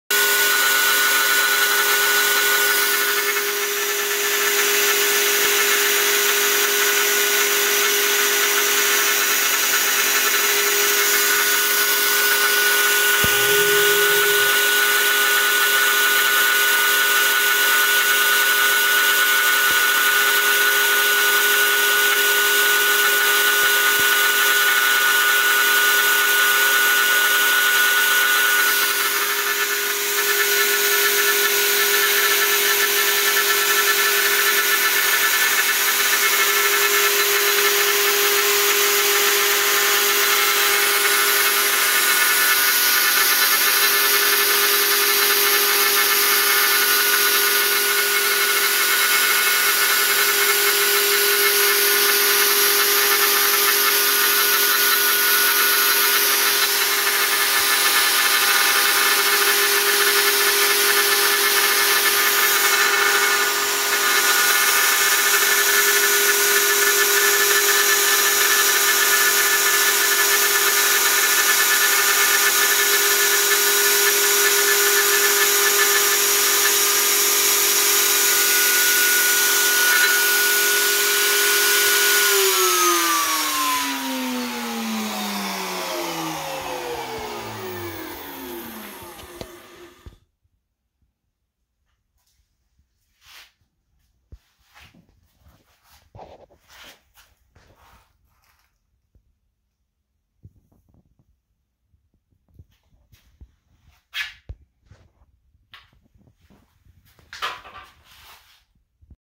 Milling aluminum - shop built slot mortiser
Just a clip. I'm honestly amazed at how well this worked. Not a perfect cut, but.. pretty goddamn okay. Especially with a two flute router bit and an XY table with no real carriage stop (just the endstops tight). Unsupported 2.5" tall angle too, which certainly didn't help.

It's extremely tempting to build real carriage stops, and also tempting to make a leadscrew and halfnut for XY. I've been meaning to swap out the table for a slotted aluminum extrusion, and this being successful makes me want to do it more.